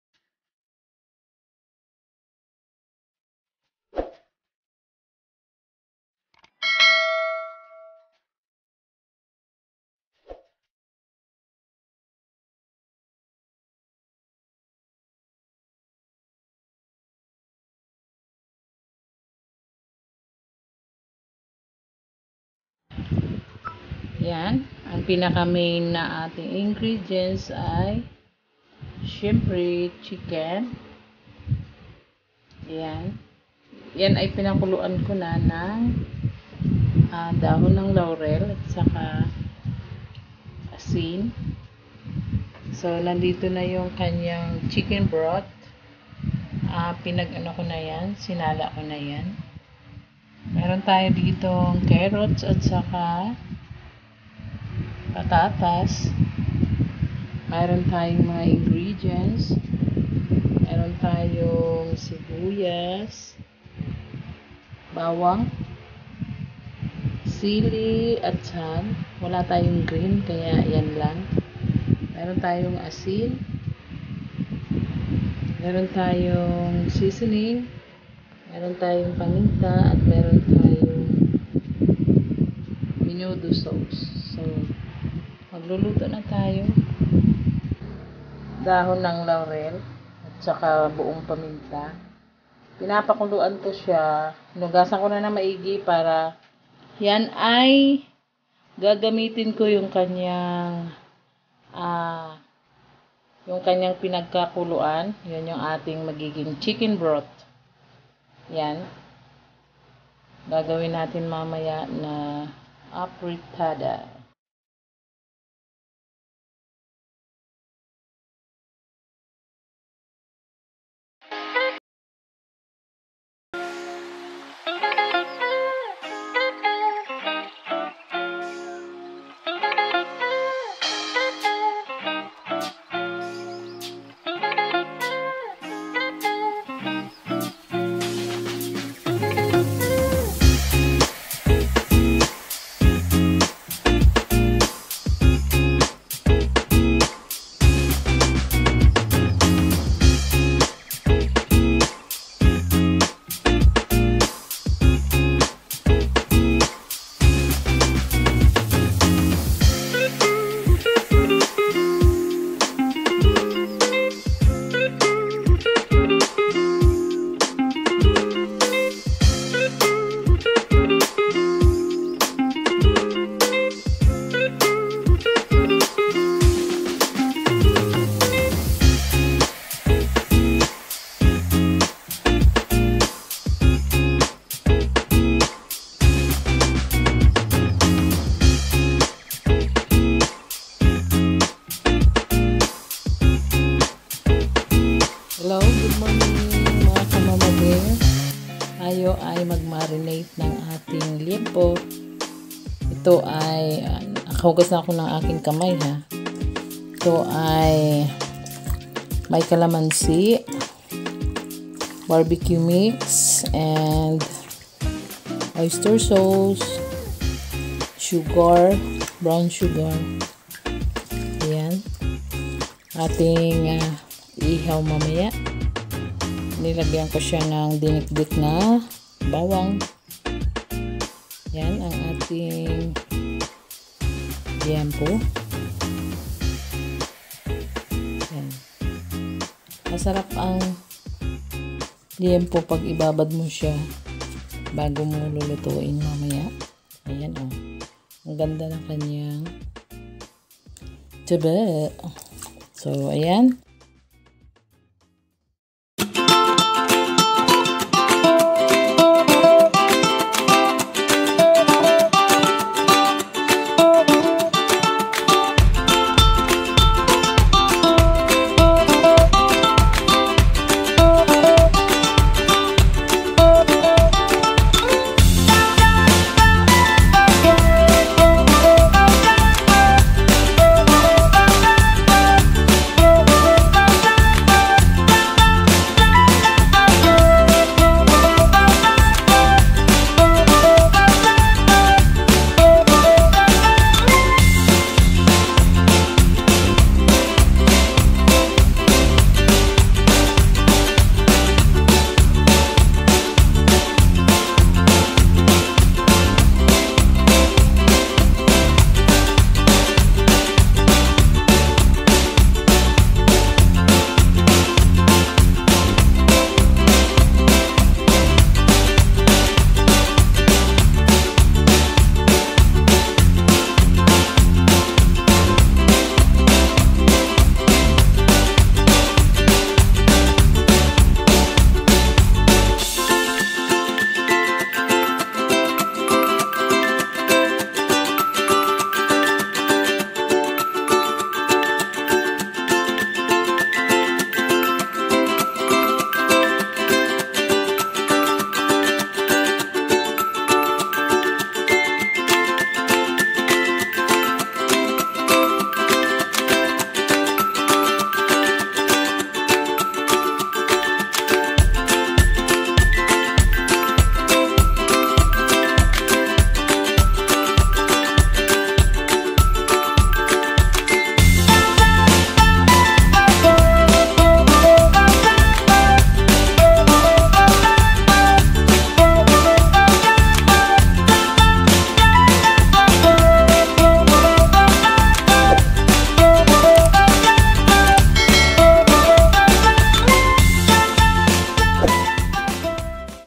Papa Bear's Birthday Celebration
Watch us as we prepare for Papa Bear's birthday. Let us enjoy and have a great time together, mga ka-Family Bear